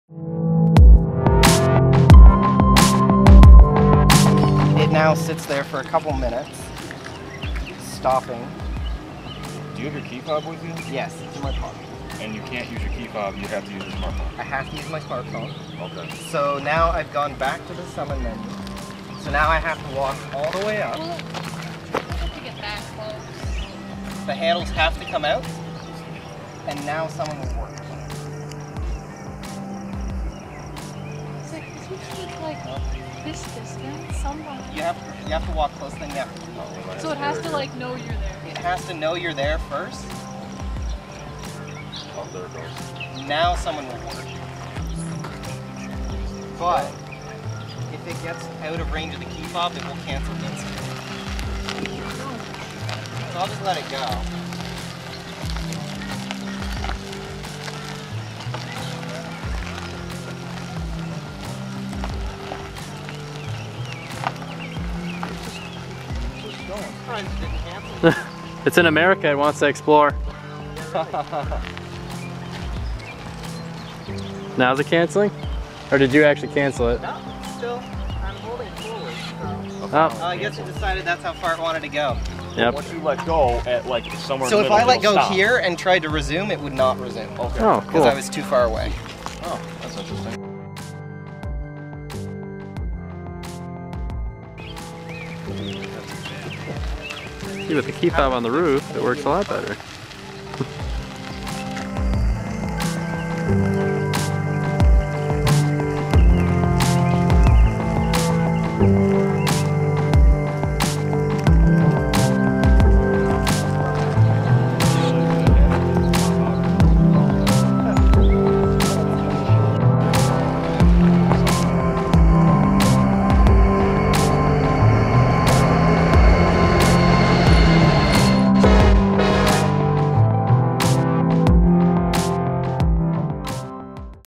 Tesla Summon - Different on a Canadian Model S?
Is Tesla Summon different on a Canadian Tesla Model S find out in this video. I must say watching the car drive itself is surreal!